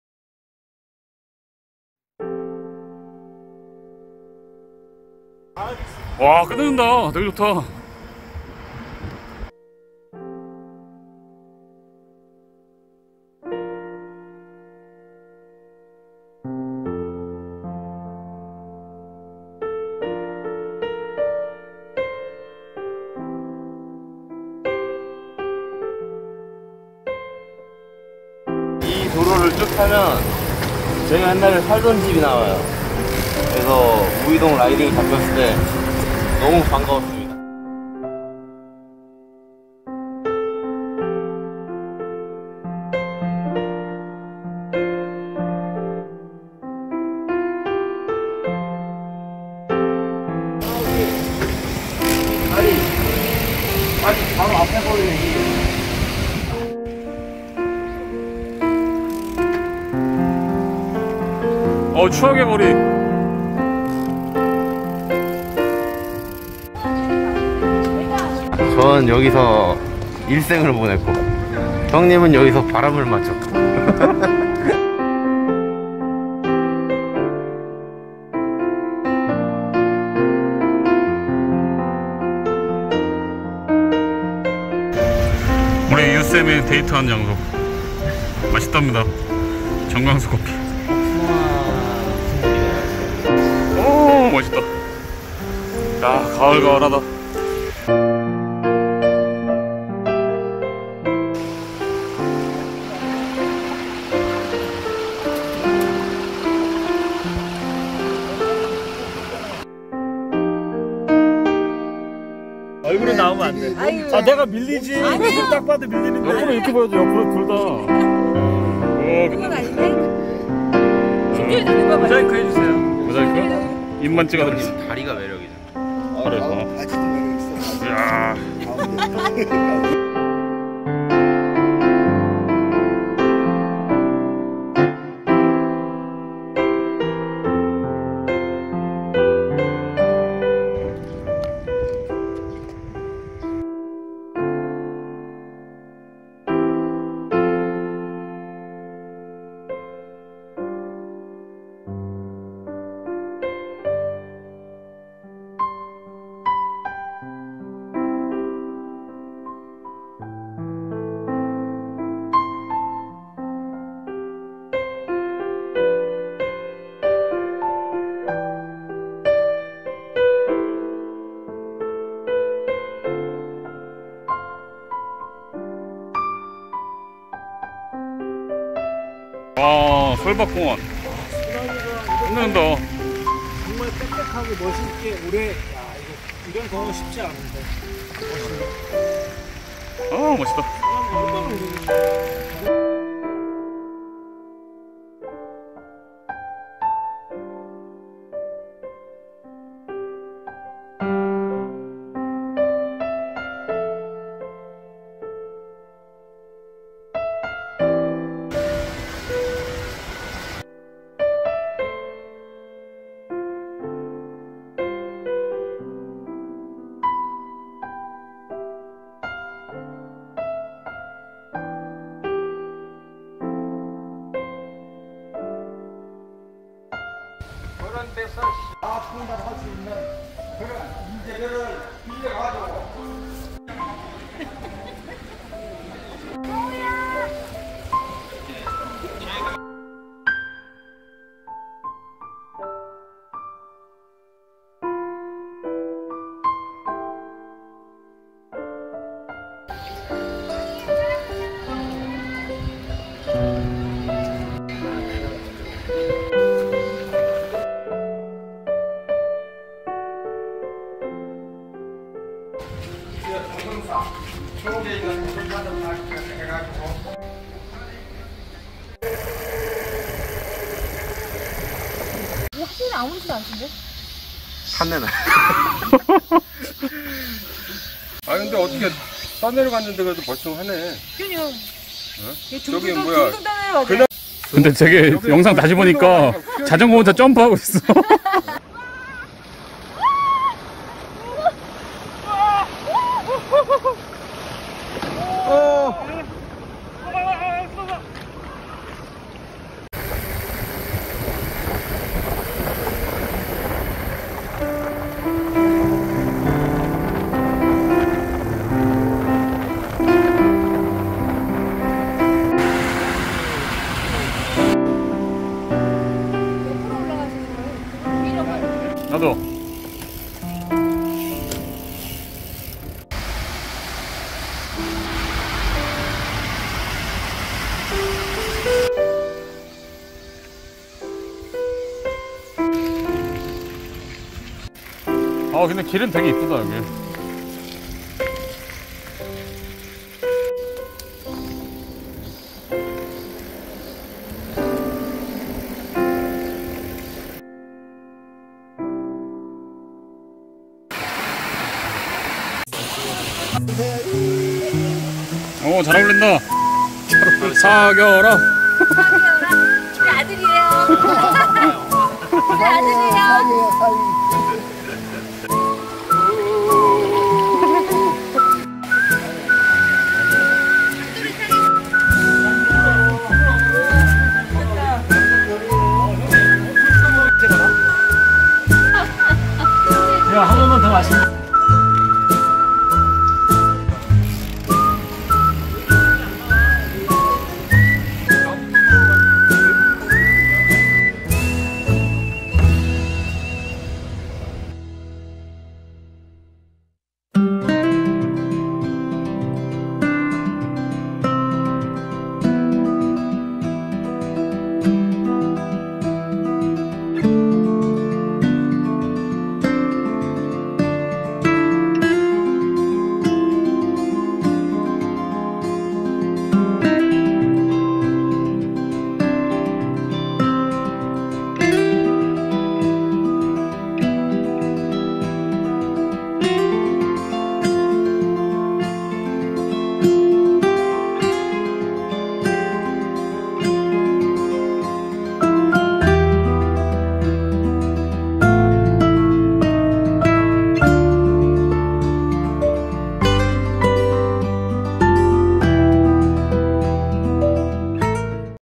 우이동 MTB 라이딩 / 북한산 / 419 묘역 / 도선사 / Uidong riding, Bukhansan, April 19th National Cemetery, Dosunsa
[이어폰으로 들으세요]
가을같이 선선한 8월말 날씨에 우이동으로 테마 라이딩 다녀왔습니다.
우이천을 따라 북한산을 향해 오르며 유쌤의 생가(?)도 투어하고
덕성여대, 북한산 주변, 419묘역, 솔밭공원, 임도, 계곡등 아기자기한 즐거움이 있는 라이딩이었습니다.

It was such a exciting MTB tour riding at Uidong. We headed Udichon after crossing Jungrangcheon. Pedaling looking at Bukhansan was another fun. Passing Duksung women univ., we stopped at April 19th national cemetery. Bukhansan and Dosunsa temple was great place to enjoy.

bgm
- yoopd music 220826